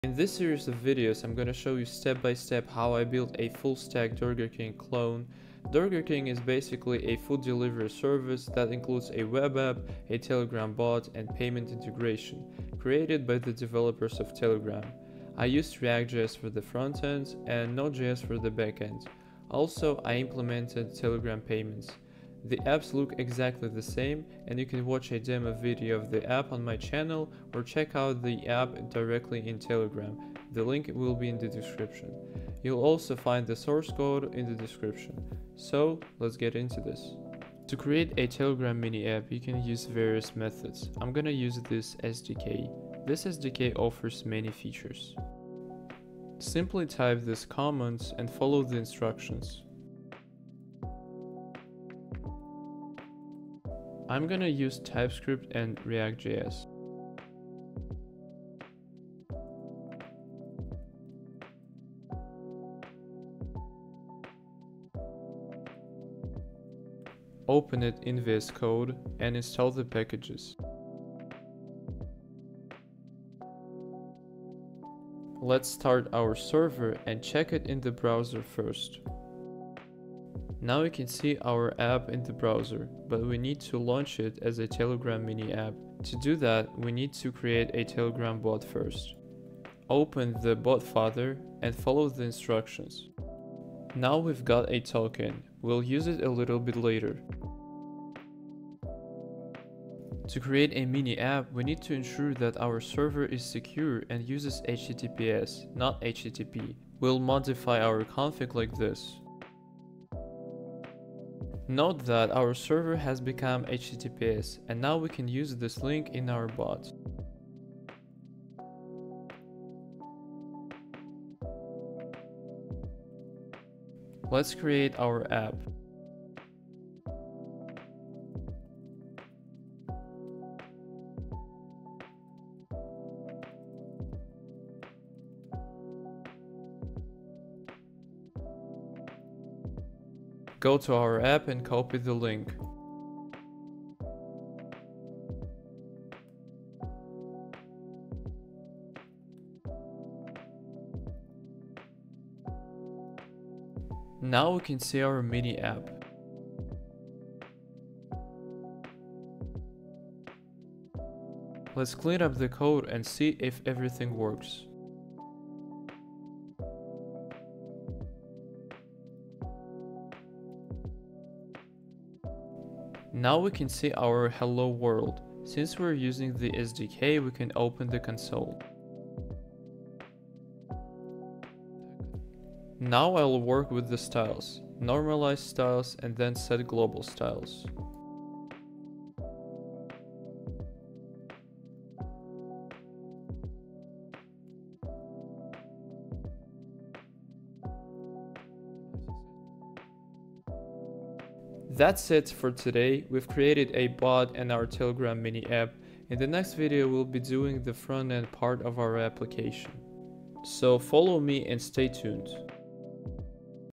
Building Durger King - Creating Bot + Telegram Mini App #1 [FULL GUIDE]
We're building a FULLSTACK Telegram Mini App Shop - Durger King
In this video, we'll create your first Telegram Mini App + Telegram Bot

Get the full app - @tgshopboilerplate
Check the full app - @tgshopboilerplate

00:00 Demo + Tech Stack
00:38 Creating project
01:26 Creating Telegram Bot
01:47 Config settings
02:06 Creating Telegram Mini App
02:33 We can see our Mini App!
03:03 Working with styles(normalize.css + global styles)